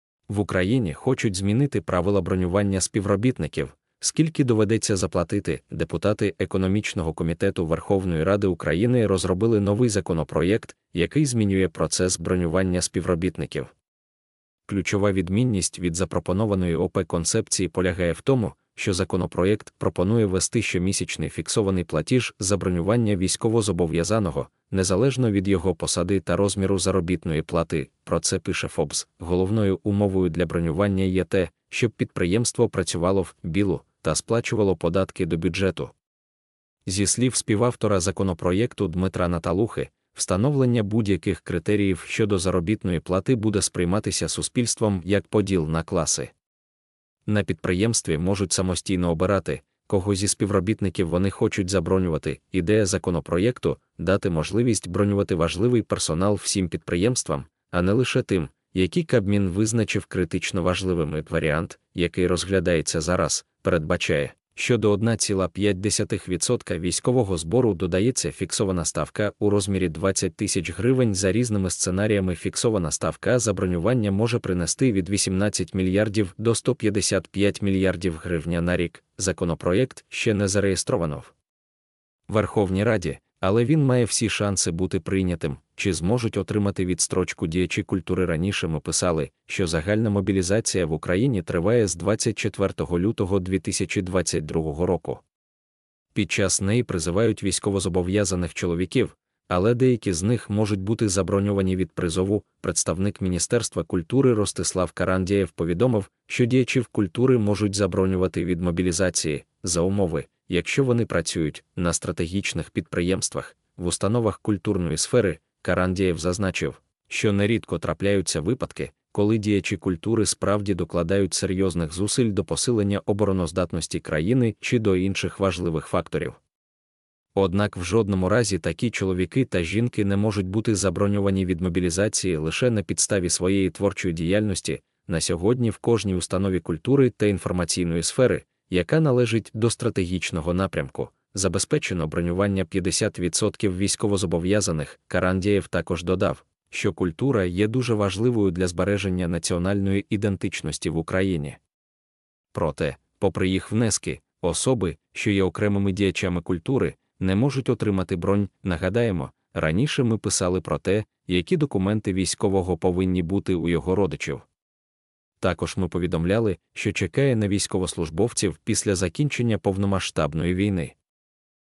В Україні хочуть змінити правила бронювання співробітників: скільки доведеться заплатити
Депутати економічного комітету Верховної Ради України розробили новий законопроєкт, який змінює процес бронювання співробітників. Ключова відмінність від запропонованої ОП концепції полягає в тому, що законопроєкт пропонує ввести щомісячний фіксований платіж за бронювання військовозобов'язаного, незалежно від його посади та розміру заробітної плати.

Про це пише Forbes.

Головною умовою для бронювання є те, щоб підприємство працювало в "білу" та сплачувало податки до бюджету.

Зі слів співавтора законопроєкту Дмитра Наталухи, встановлення будь-яких критеріїв щодо заробітної плати буде сприйматися суспільством як поділ на класи. На підприємстві можуть самостійно обирати, кого зі співробітників вони хочуть забронювати.

Ідея законопроєкту – дати можливість бронювати важливий персонал всім підприємствам, а не лише тим, які Кабмін визначив критично важливими.

Варіант, який розглядається зараз, передбачає, що до 1,5% військового збору додається фіксована ставка у розмірі 20 000 грн. За різними сценаріями фіксована ставка за бронювання може принести від 18 млрд до 155 млрд грн на рік.

Законопроєкт ще не зареєстровано в Верховній Раді, але він має всі шанси бути прийнятим.

Чи зможуть отримати відстрочку діячі культури

Раніше ми писали, що загальна мобілізація в Україні триває з 24 лютого 2022 року. Під час неї призивають військовозобов'язаних чоловіків, але деякі з них можуть бути заброньовані від призову.

Представник Міністерства культури Ростислав Карандєєв повідомив, що діячів культури можуть забронювати від мобілізації, за умови, якщо вони працюють:

на стратегічних підприємствах;

в установах культурної сфери.

Карандєєв зазначив, що нерідко трапляються випадки, коли діячі культури справді докладають серйозних зусиль до посилення обороноздатності країни чи до інших важливих факторів. Однак в жодному разі такі чоловіки та жінки не можуть бути заброньовані від мобілізації лише на підставі своєї творчої діяльності.

На сьогодні в кожній установі культури та інформаційної сфери, яка належить до стратегічного напрямку, забезпечено бронювання 50% військовозобов'язаних.

Карандєєв також додав, що культура є дуже важливою для збереження національної ідентичності в Україні. Проте, попри їх внески, особи, що є окремими діячами культури, не можуть отримати бронь.

Нагадаємо, раніше ми писали про те, які документи військового повинні бути у його родичів. Також ми повідомляли, що чекає на військовослужбовців після закінчення повномасштабної війни.